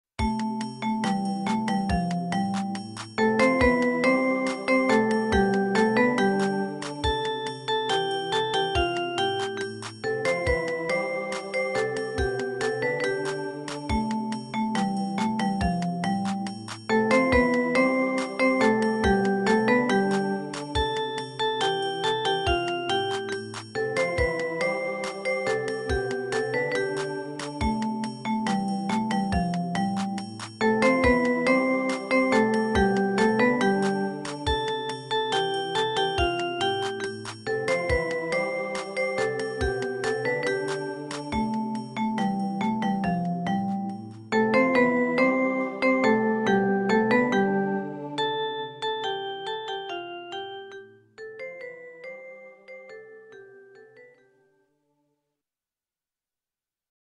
String technique used on Vibraphone and Echo pad...
Also added a sort of weird drum track pattern, but it seemed to work.
Just tried to keep it in the key of C major...